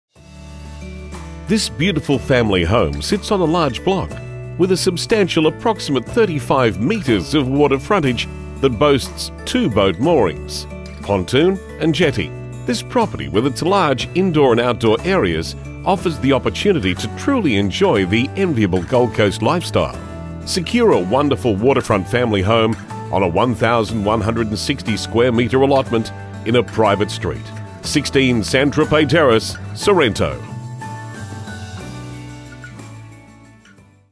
16 St Tropez Terrace Bundall 4217 QLD
This beautiful family home sits on a large block with a substantial 35m* of water frontage that boasts two boat moorings - Pontoon & Jetty. This property with its large indoor and outdoor areas offers the opportunity to truly enjoy the enviable Gold Coast lifestyle. Secure a wonderful waterfront family home on a 1,160m2* allotment in a private street!16 St Tropez Terrace, Sorrento